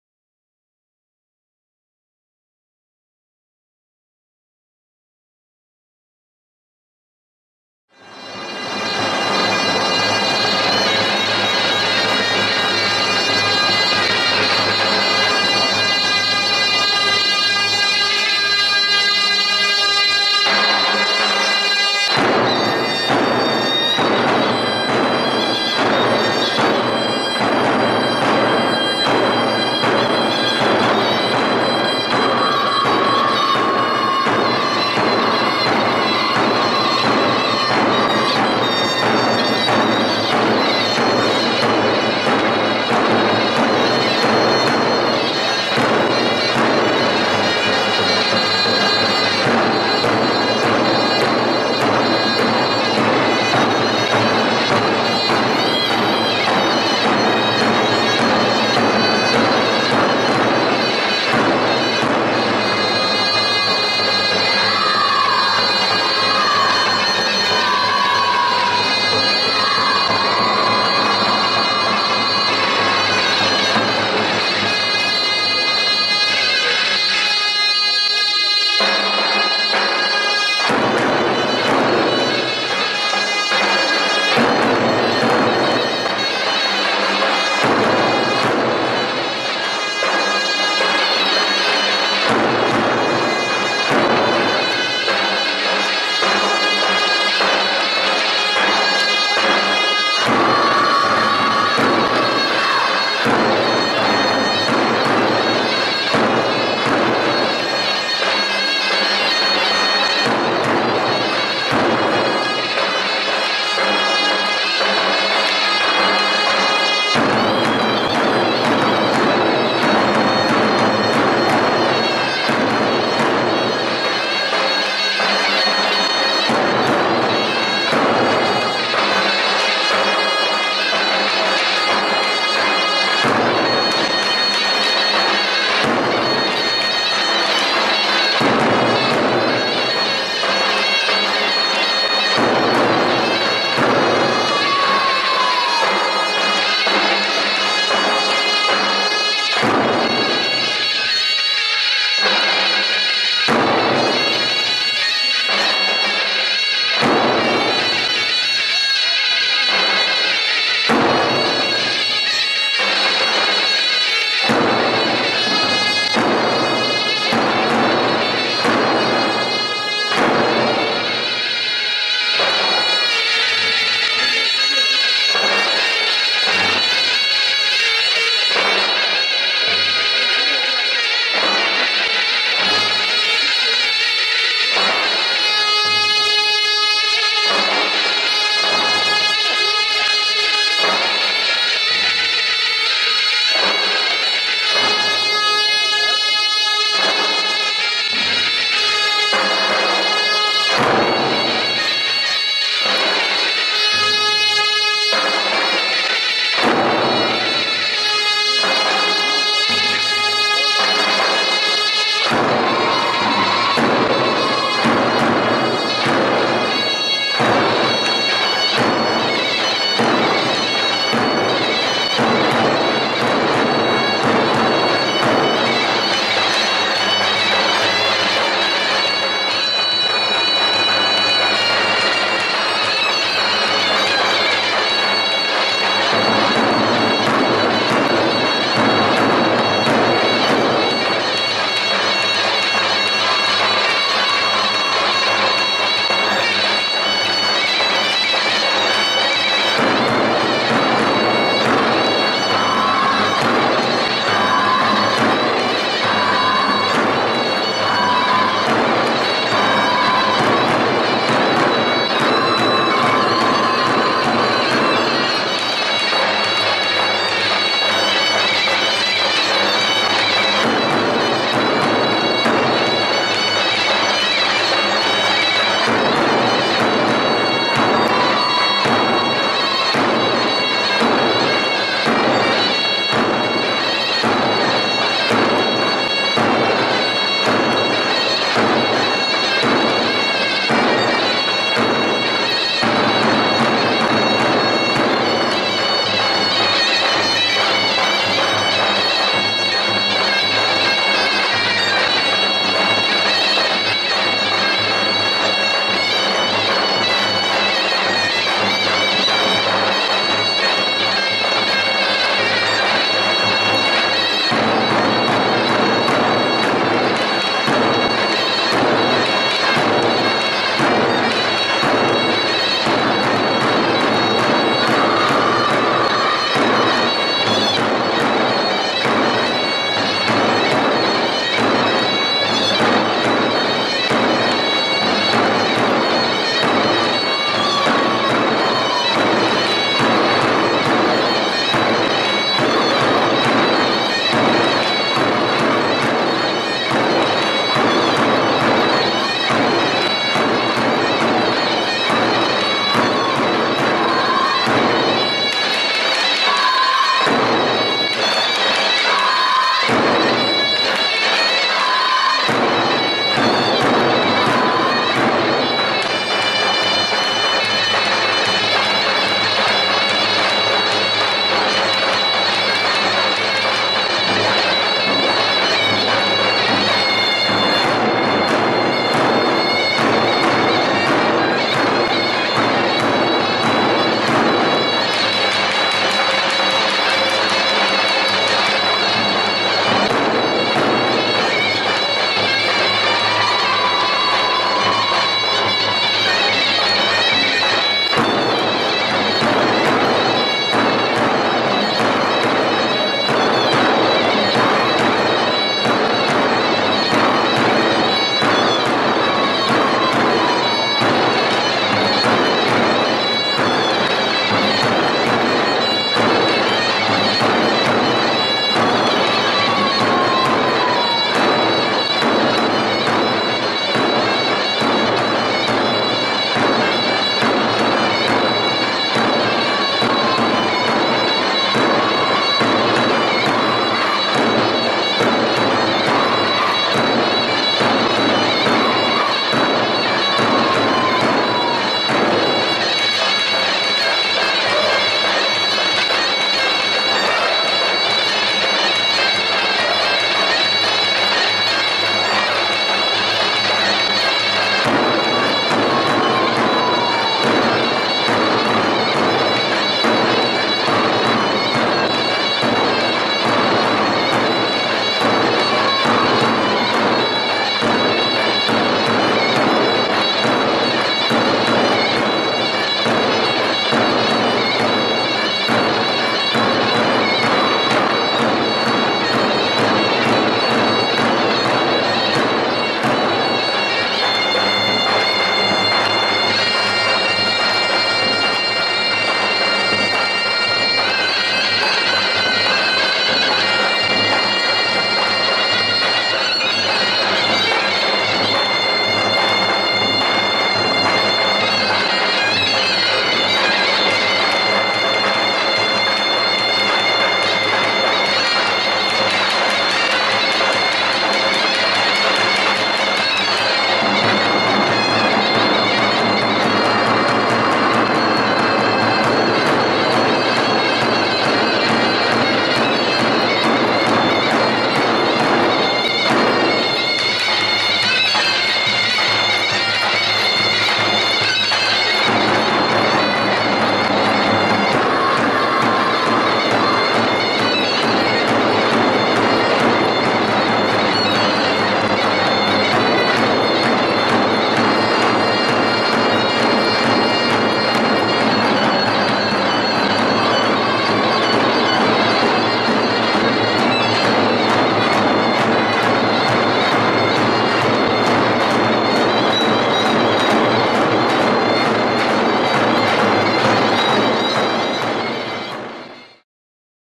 Şehitkamil Merveşehir Halk Eğitim Merkezi SK. / Kulüpler Halk Oyunları İl Yarışması-Gaziantep-2022
Minikler Geleneksel Düzenlemesiz Dal